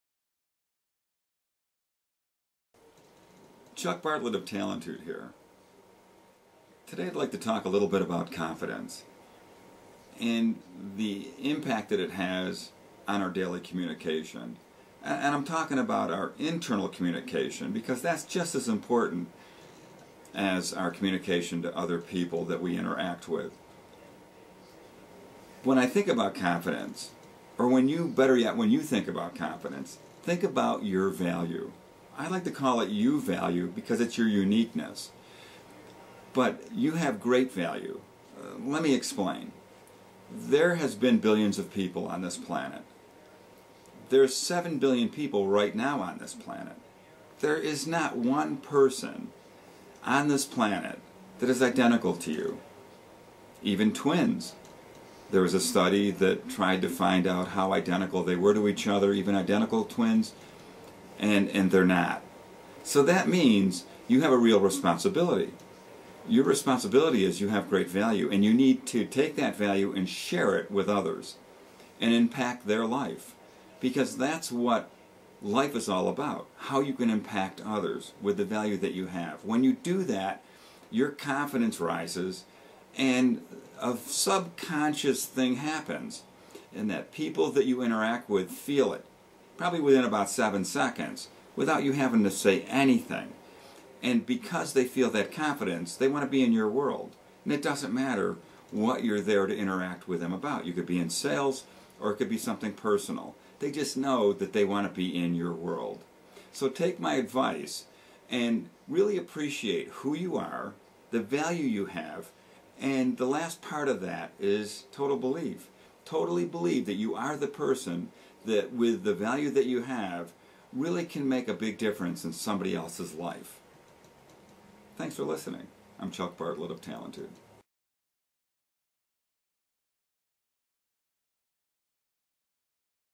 Confidence In Yourself - Chuck Bartlett of Talentude™
Confidence In Yourself - Chuck Bartlett of Talentude, shares a very easy way on how to look at how confidence in yourself can lead you to success.